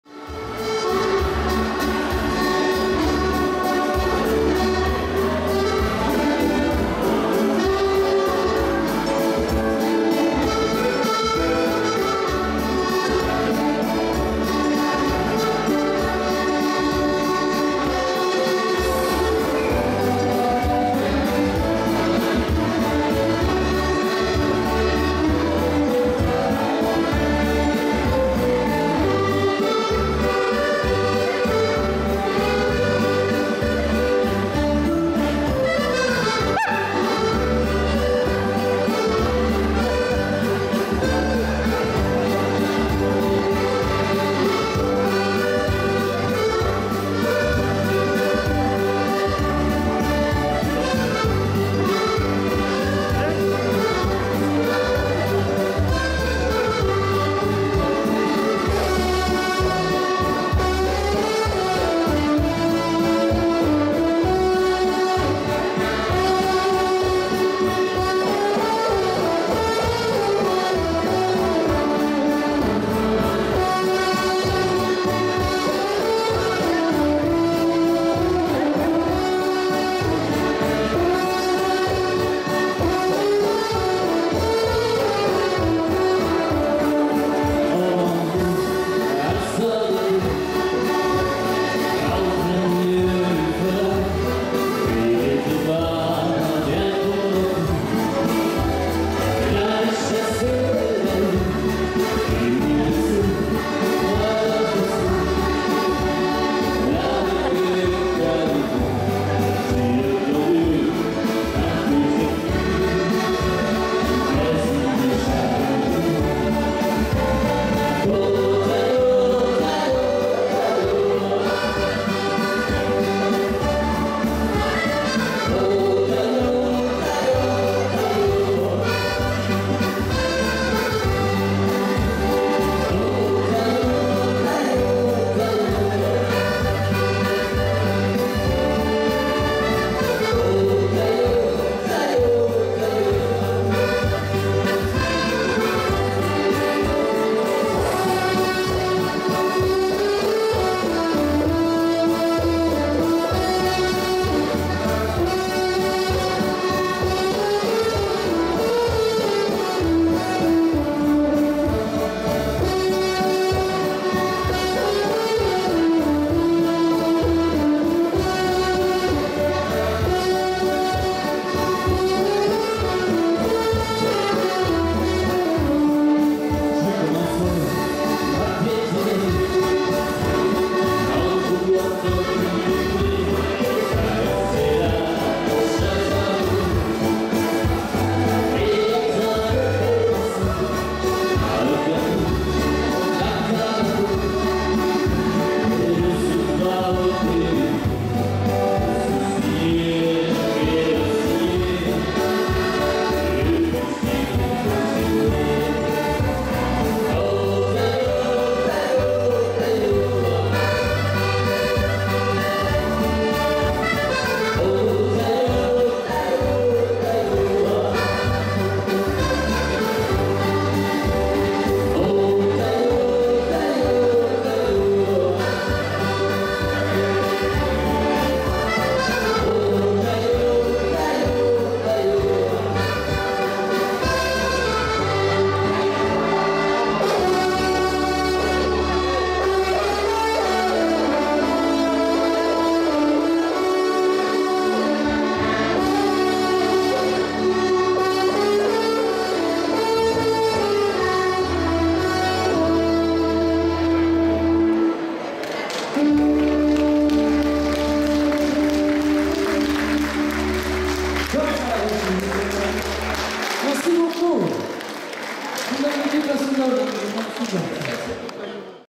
Quentin LAROCHE Tolerme juin 2023 Valse accordéon et trompe de chasse
Quentin Laroche au lac de Tolerme (46) chez Gérard et Christiane Lacaze - vidéo et réalisation Alexandra Astorg (Sandy)